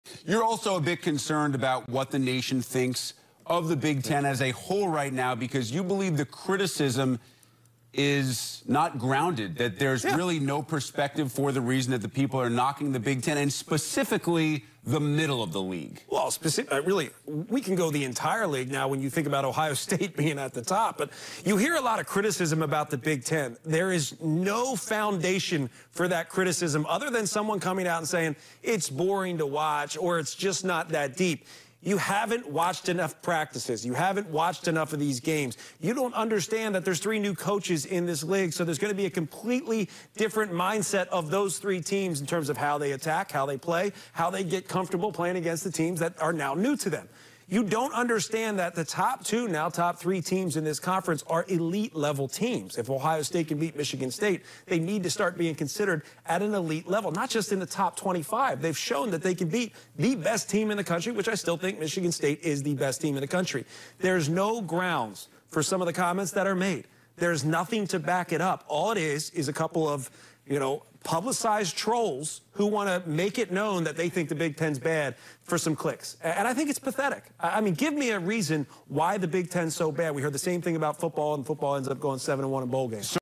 Jon Crispin on Unfair Criticism of B1G
BTN basketball analyst Jon Crispin talks about the unfair criticism directed at the Big Ten.